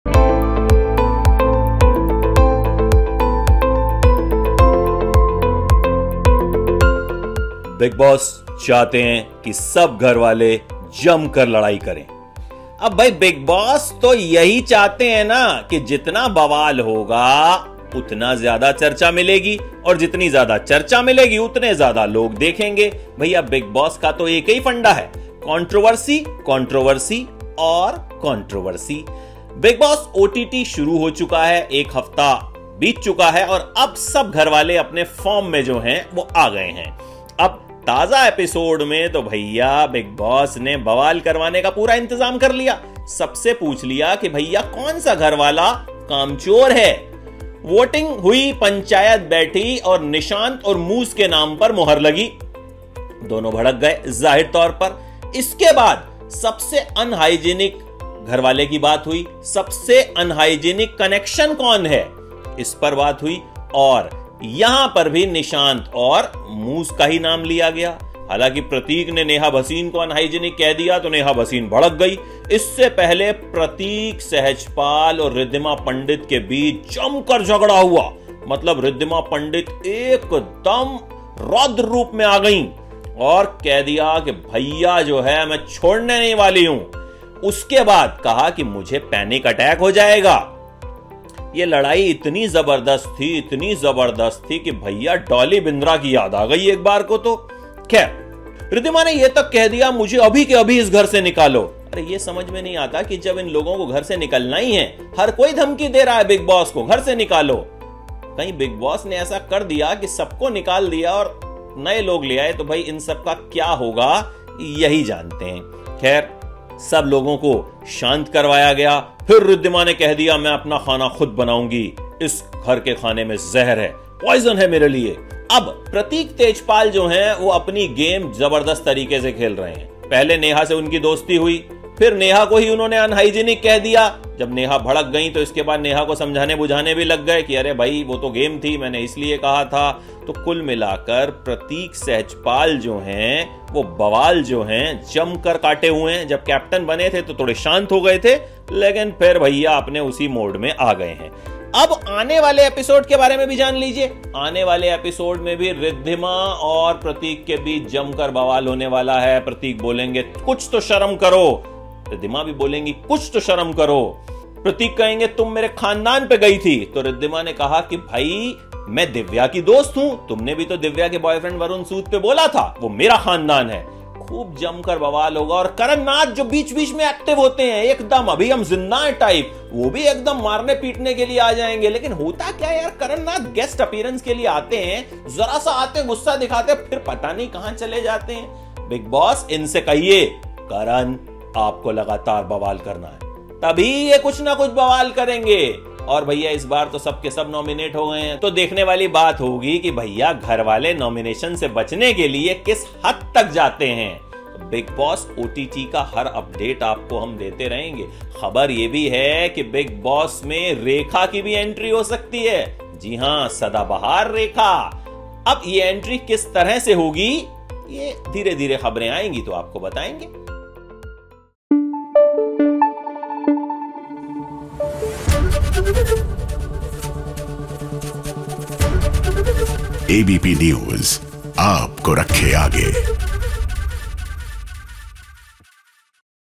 Bigg Boss 15 OTT: Pratik Sehajpal की ज़बरदस्त लड़ाई ! Rekha की एंट्री हो सकती है?
बिग बॉस ओटीटी इन दिनों काफी चर्चा में बना हुआ है. हर दिन घर में हो रहे लडाई-झगड़े फैन्स को खूब एंटरटेन कर रहे हैं.